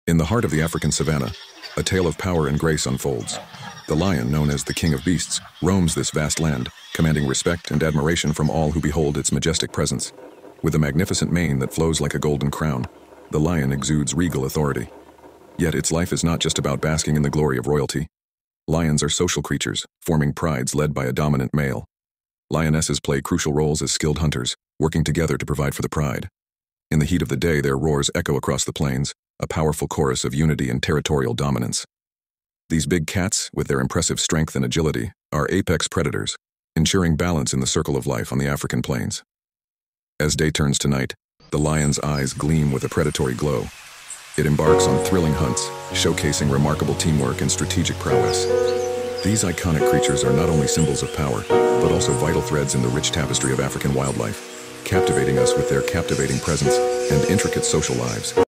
The Lion King | The Incredible World of Lions
The majestic lion, king of the jungles, is one of nature's most fascinating creatures. With its powerful claws and impressive mane, it reigns supreme over vast African lands. However, a lion's life is much more than just royalty and power.

Lions are social animals, living in groups known as "prides," where lionesses play vital roles in hunting and raising cubs. Their vocalizations resonate across the savannah, communicating and maintaining group cohesion.

Furthermore, lions' incredible strength and agility are showcased in their hunts, where they can reach astonishing speeds in short bursts of movement. As top predators, they play a crucial role in balancing African ecosystems.

The lion is more than just a symbol of power; it is a vital piece of the African wildlife puzzle, a creature that continues to captivate us with its captivating beauty and behavior.

goodbye